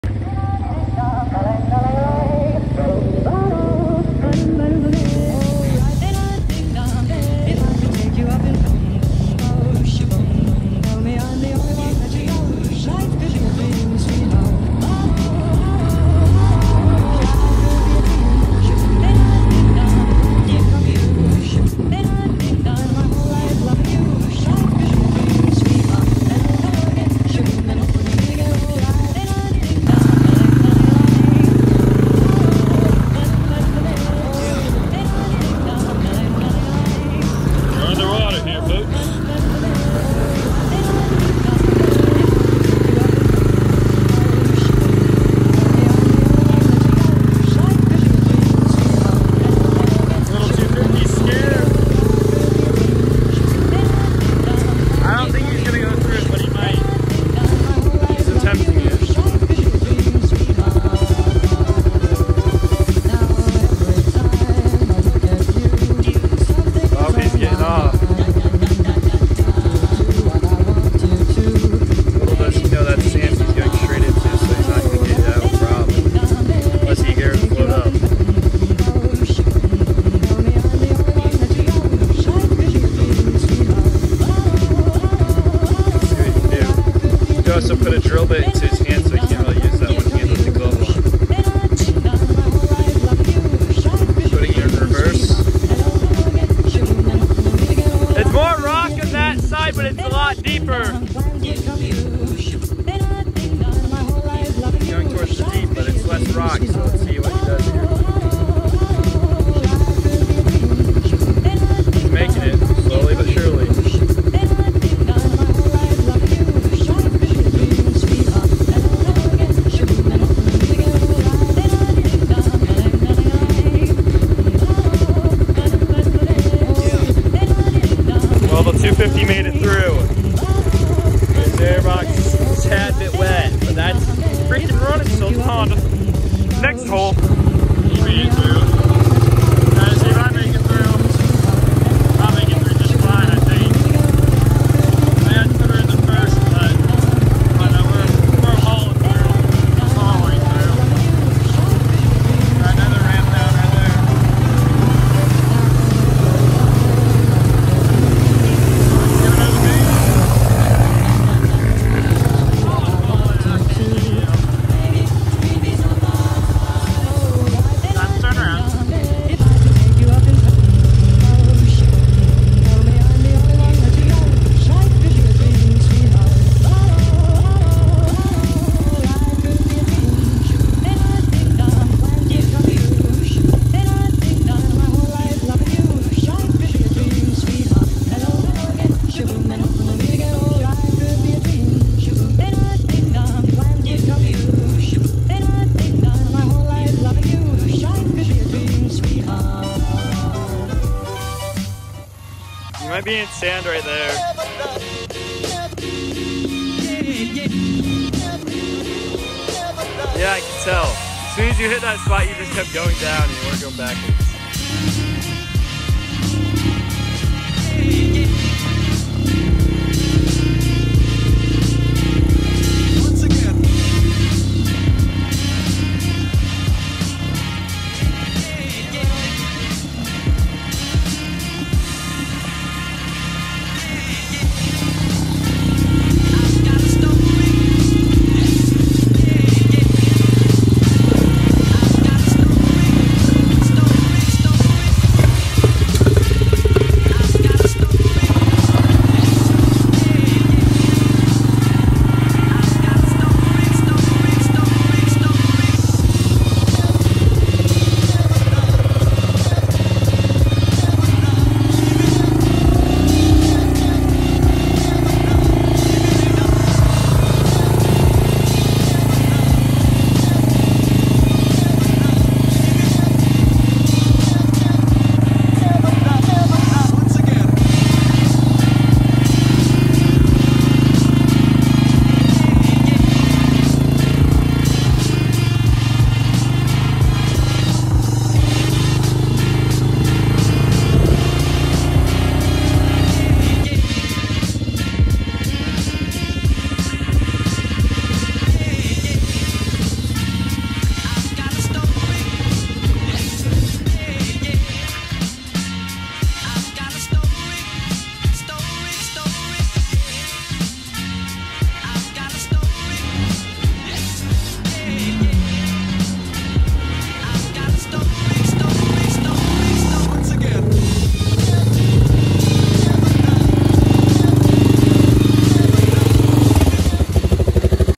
Hondas Go HOLE for HOLE 250 VS 300
Me and my cousin go hole for hole on our hondas! hope you enjoy the video almost to 1000 subscribers let make it happen on this one!!!